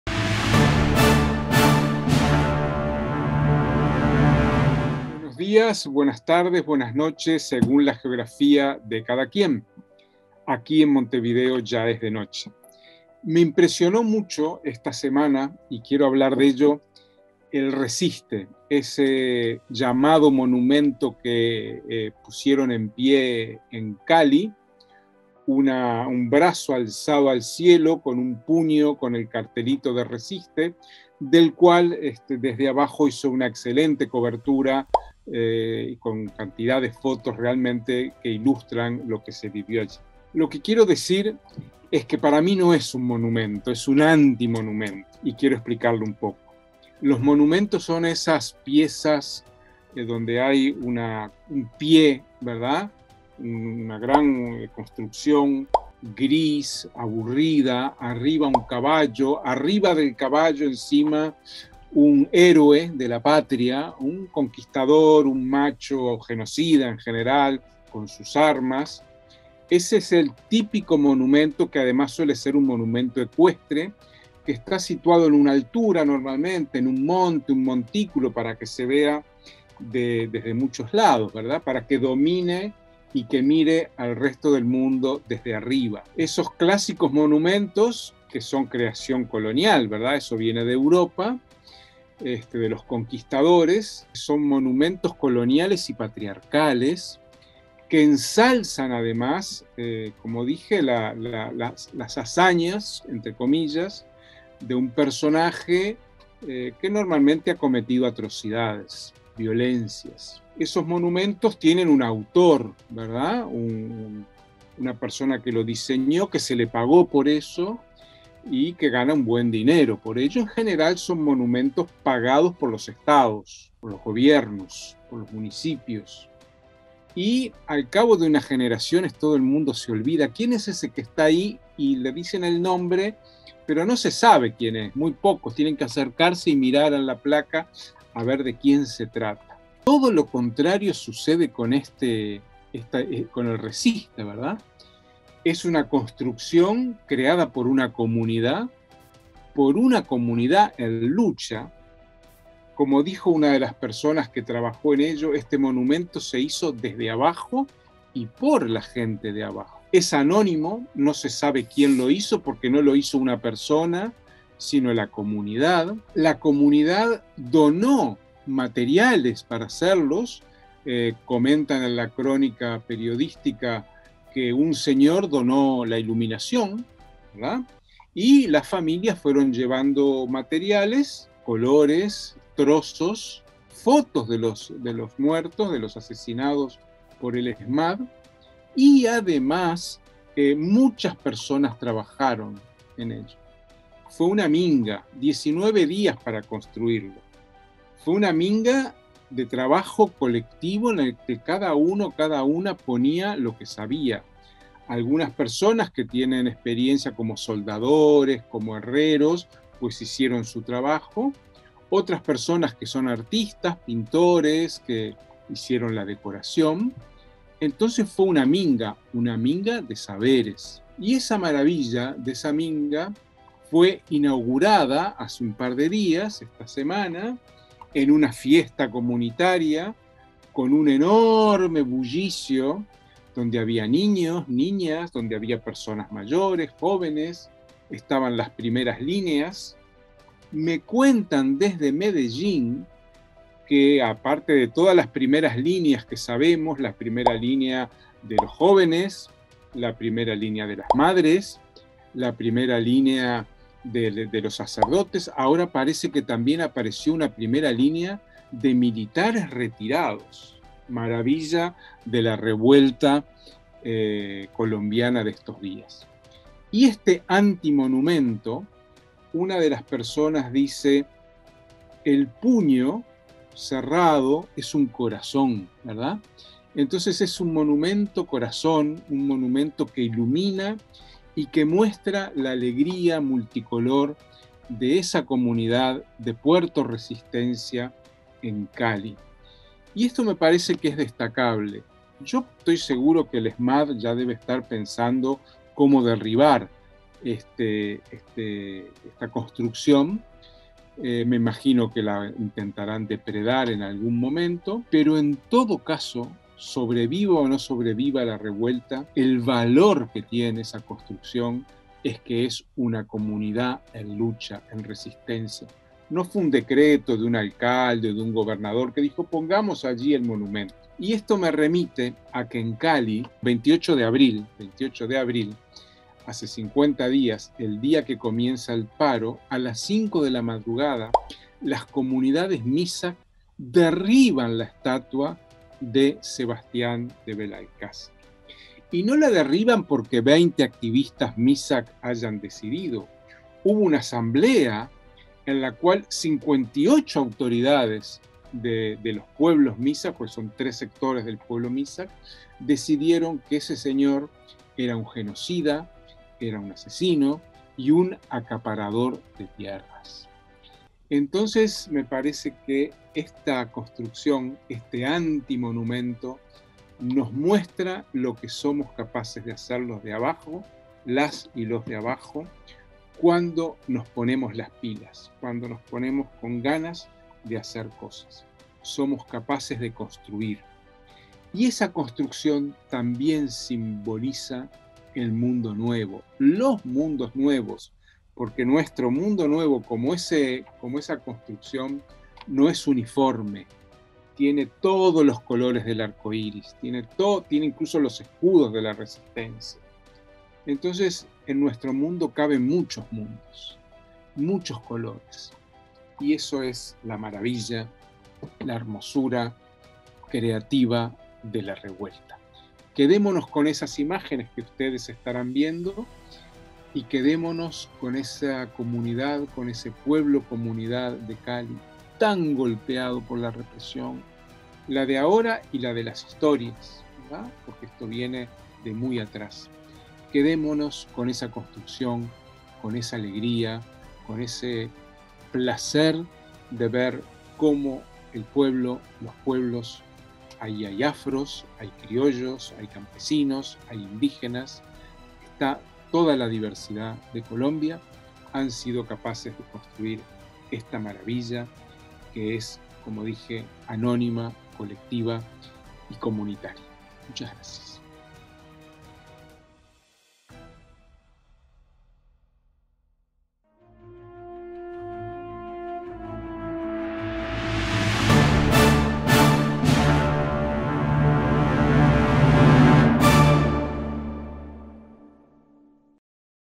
La dosis Nº67: En Cali la potencia de un antimonumento
La comunidad Puerto Resistencia, en Cali, tras semanas de lucha también toma la iniciativa en el campo cultural, al inaugurar en días pasados un monumento, una construcción de 12 metros de altura, que forma un brazo que se extiende hacia el cielo y culmina con un puño que sostiene un cartel con el lema Resiste.